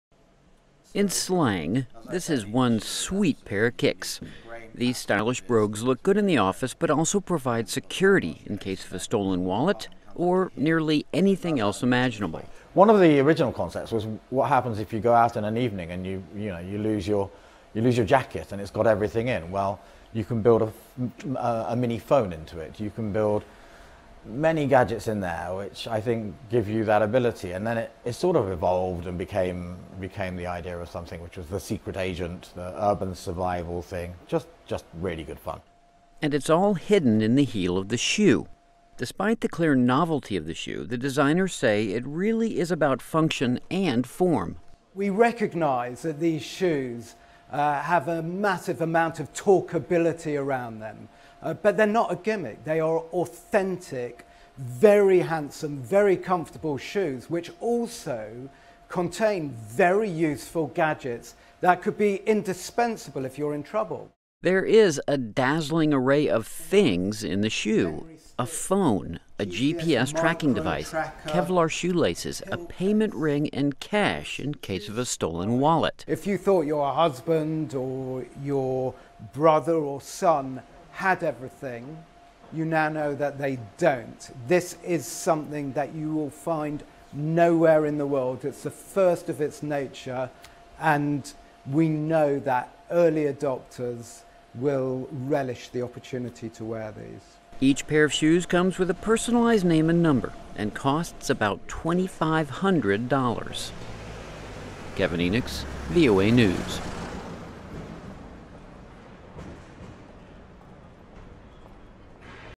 A Shoe James Bond Would Be Proud Of
For every person who just loves James Bond, it's the perfect gift. A very nice, stylish pair of shoes, that just happen to be full of high-tech hidden compartments. VOA's Kevin Enochs reports.